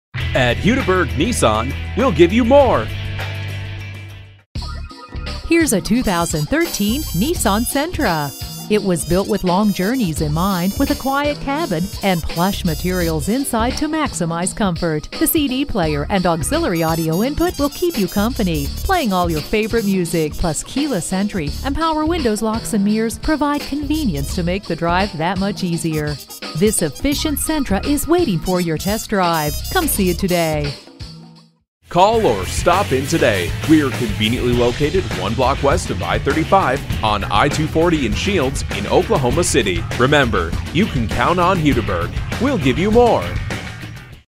2013 Nissan SENTRA Oklahoma-City OK Norman OK Tulsa, OK #25695 - SOLD
Year: 2013
Make: Nissan
Model: SENTRA
Trim: SV
Engine: 1.8 Liter 4 Cylinder
Transmission: Automatic
Color: Super black
Interior: Black
Stock #: 25695

Address:
I-240 & S Shields